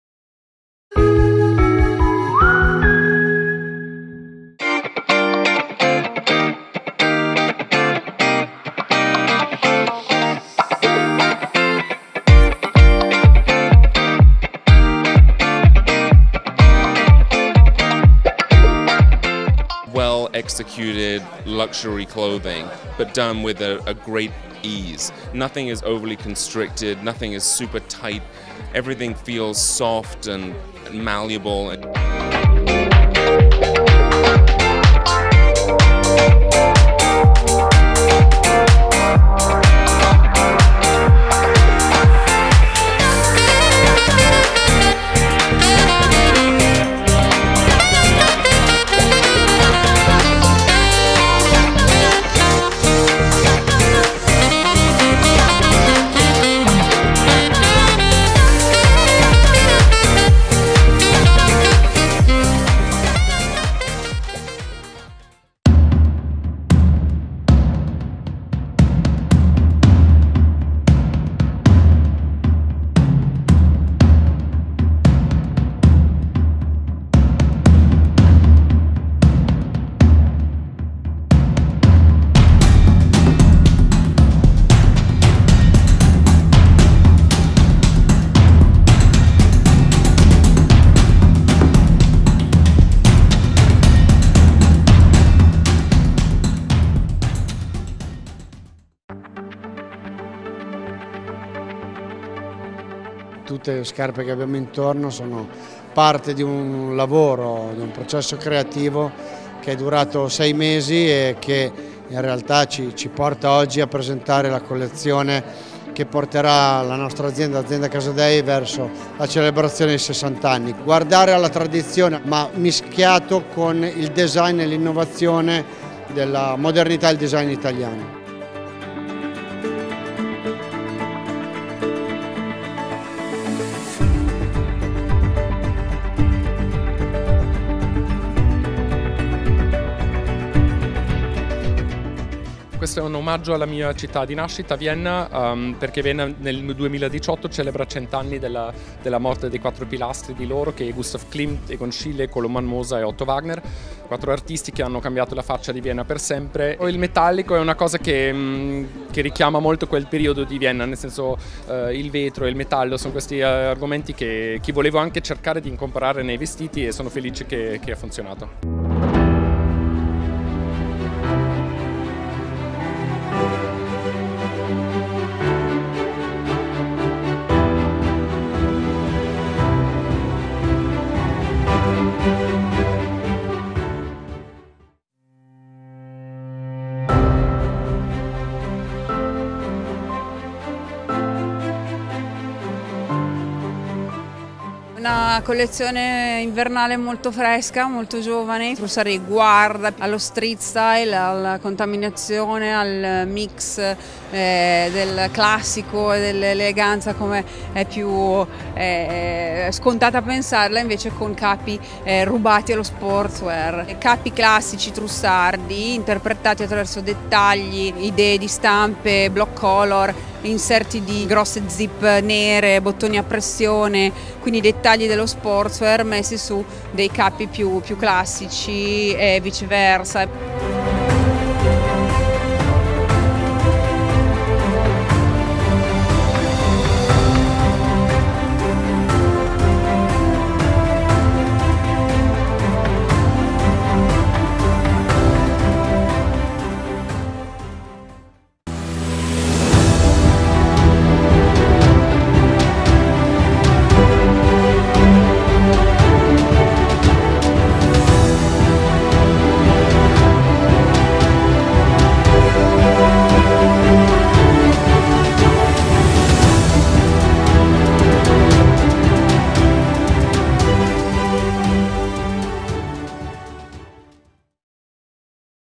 Milano Fashion Week: le 10 collezioni top viste in passerella
Si è conclusa la Settimana della Moda di Milano, sulle cui passerelle sono state presentate le collezioni donna per il prossimo Autunno/Inverno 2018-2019 dei grandi brand della moda italiana ed internazionale. Da Salvatore Ferragamo a Moschino, da Trussardi ad Alberta Ferretti, passando per Versace, Fendi, Casadei e Tommy Hilfiger ecco gli show più spettacolari, le interviste agli stilisti e le 10 collezioni più belle che abbiamo visto sulle passerelle milanesi.